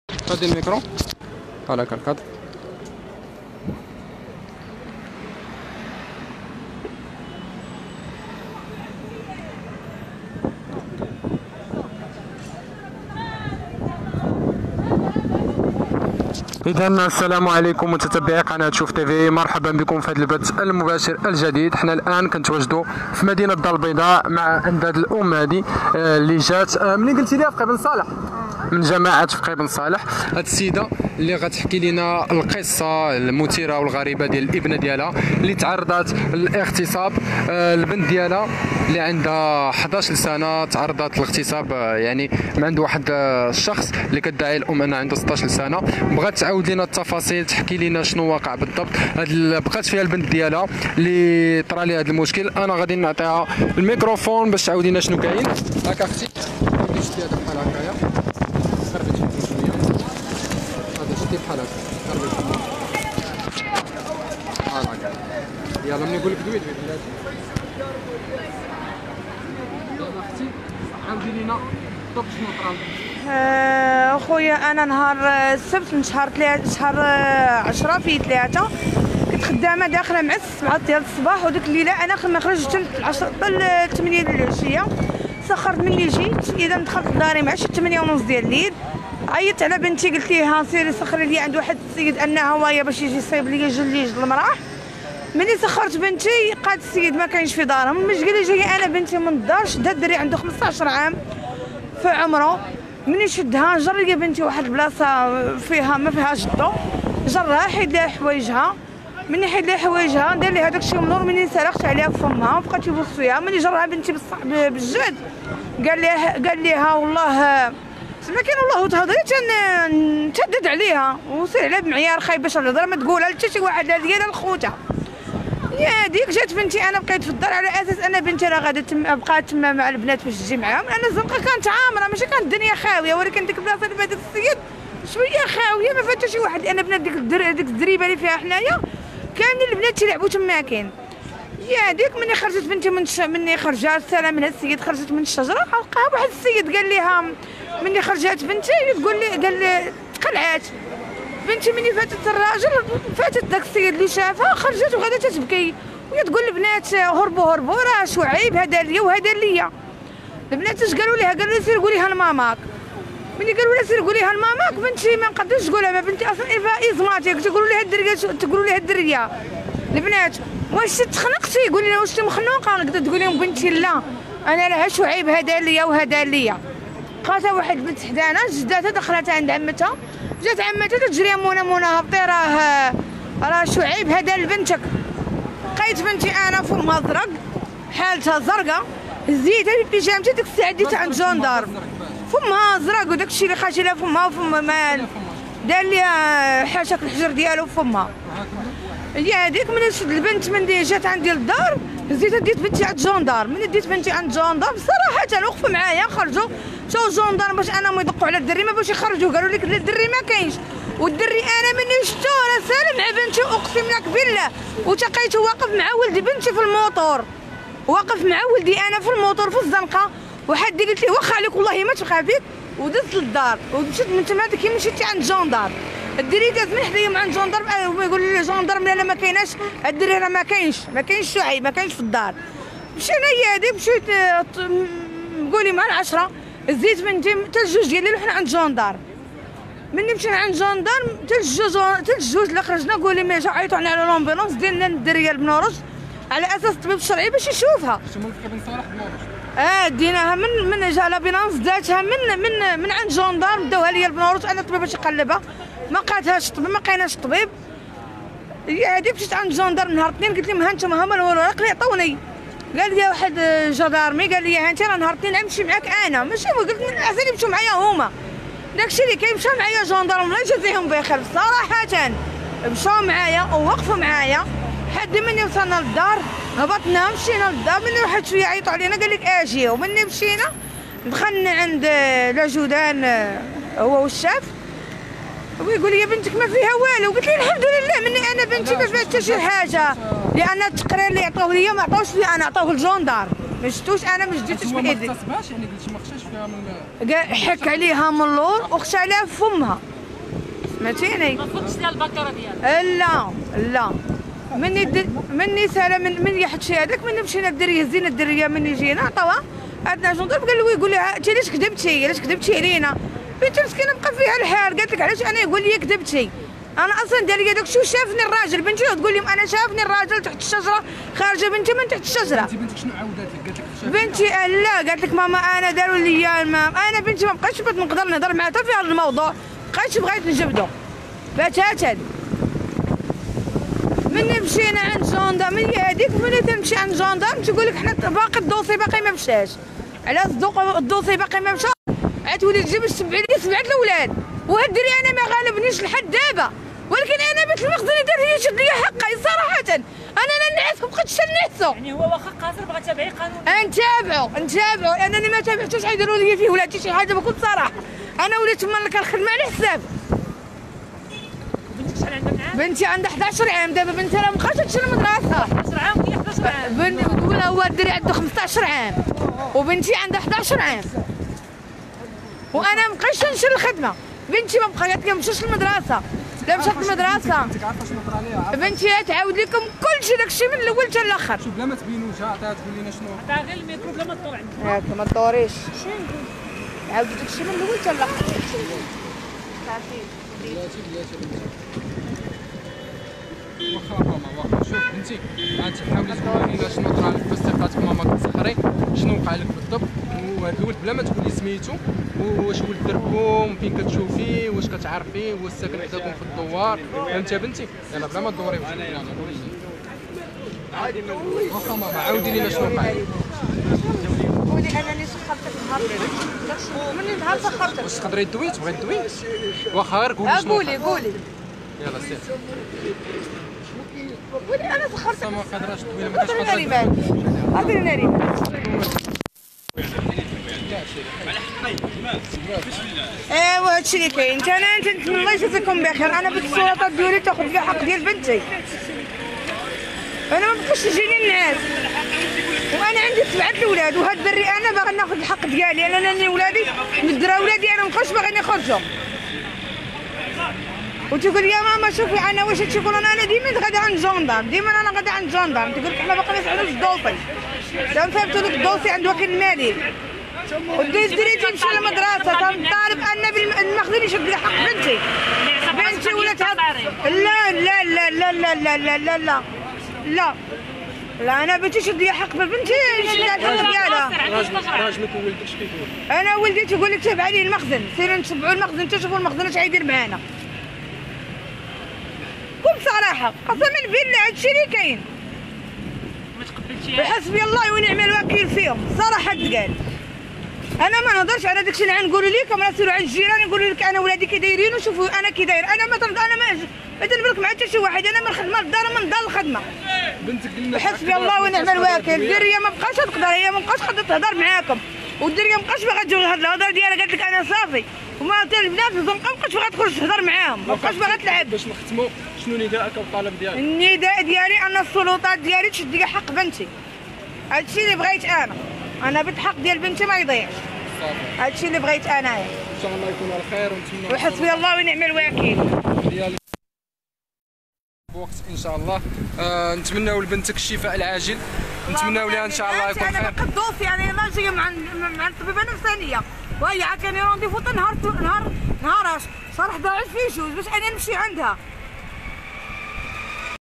قاصر عندو 16 سنة غتاصب طفلة عندها 11 سنة وداكشي لي دار فيها خطير بزاف والأم تفجرها بالدموع
مباشرة من الدار البيضاء..القضية الأخطر:قاصر عندو 16 سنة غتاصب طفلة عندها 11 سنة وداكشي لي دار فيها خطير بزاف والأم تفجرها بالدموع